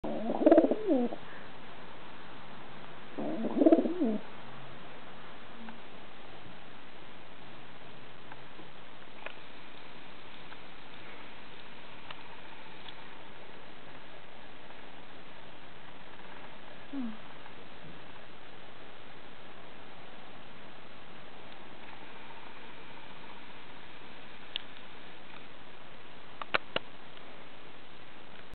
Pet pigeon (Georgie) snuggling up to me
My disabled, tame pigeon settles down for a nap. :) More about pet pigeons